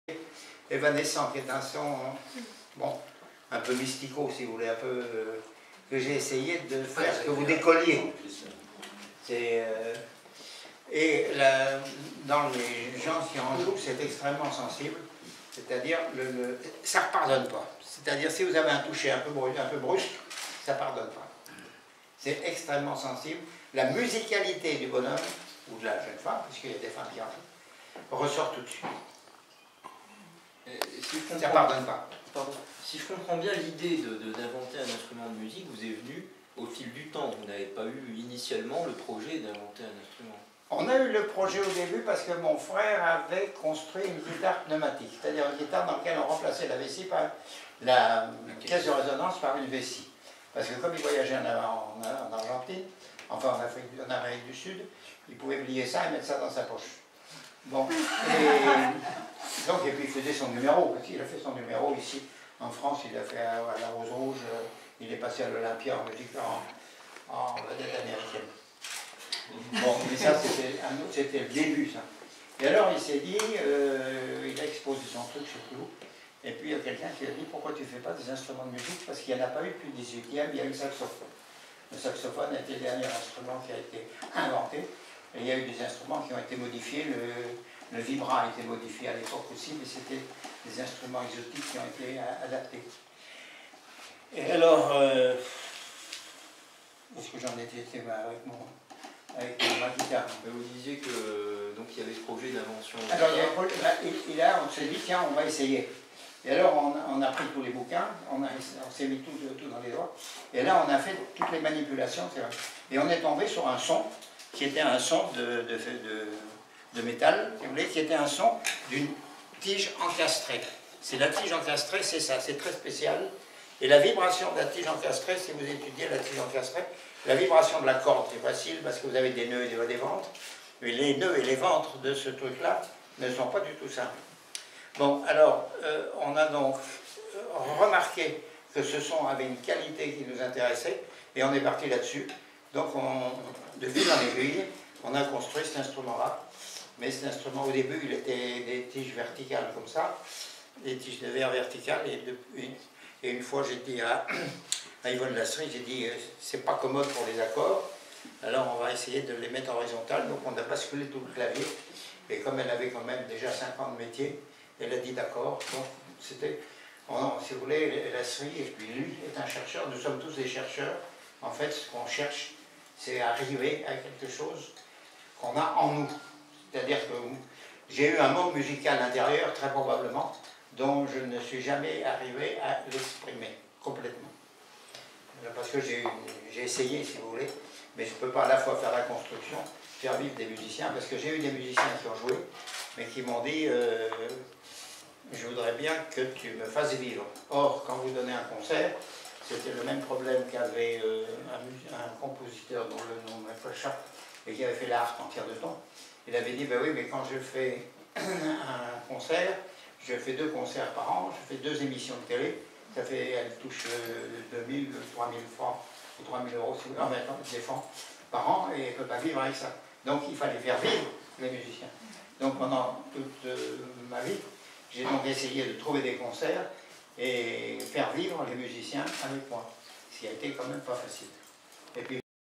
Lise CORMERY & Bernard BASCHET sur le Cristal BASCHET
Galerie Lise CORMERY où Bernard BASCHET parle de sa création du Cristal BASCHET

Plan 56 Baschet raconte OUI